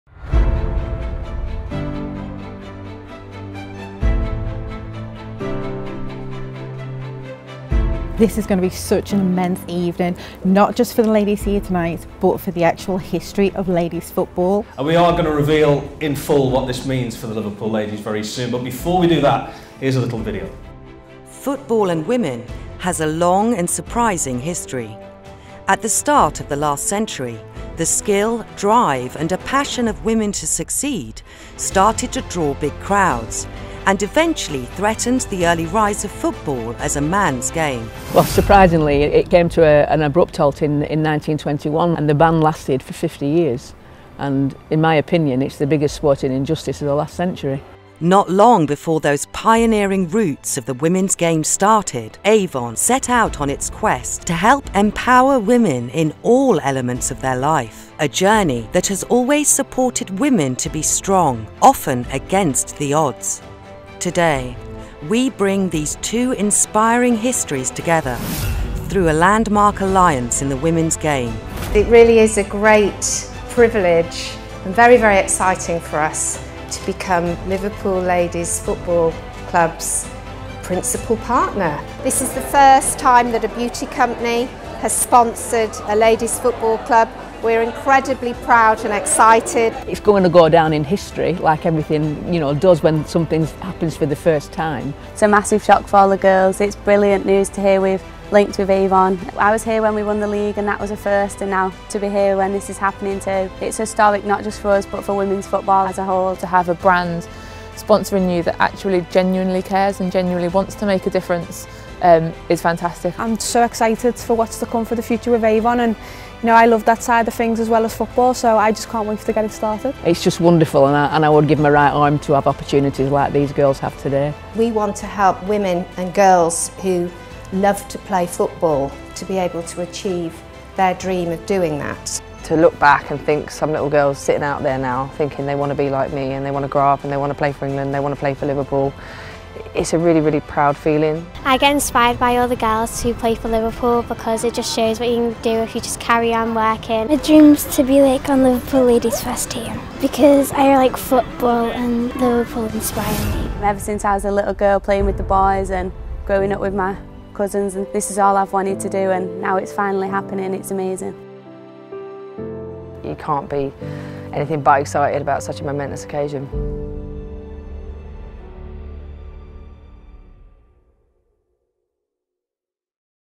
Avon announces partnership with Liverpool Ladies Football Club
As the the first female brand to sponsor an FA Women’s Super League club we aim to inspire women all over the world breaking down social barriers, challenging stereotypes, and encourage young girls and women around the world to achieve their ambitions.